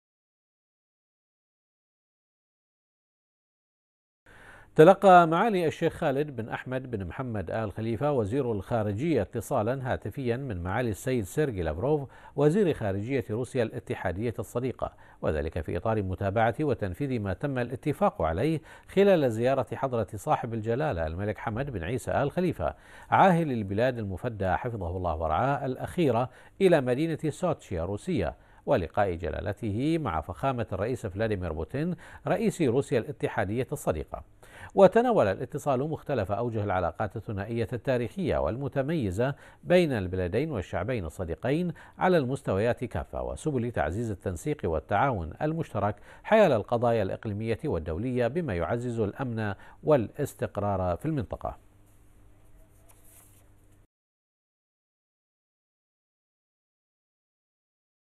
البحرين : معالي وزير الخارجية يتلقى اتصالا هاتفيا من وزير خارجية روسيا الاتحادية الصديقة
Description

معالي وزير الخارجية يتلقى اتصالا هاتفيا من وزير خارجية روسيا الاتحادية الصديقة